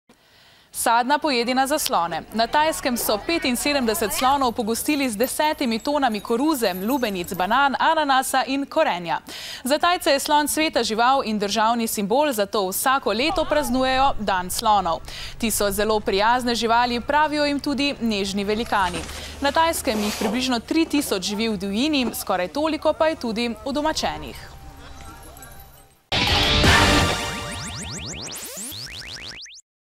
Infodrom: Sadna pojedina za slone
V starodavnem mestu Ayuthaya vsako leto 13. marca pripravijo pravo pojedino za tamkajšnje slone, ki vsebuje raznoliko sadje, koruzne storže ter drugo zelenjavo, ki je pogodu živalim. S tem se veliki živali poklonijo, javnost pa opozarjajo na problem izumiranja slonov.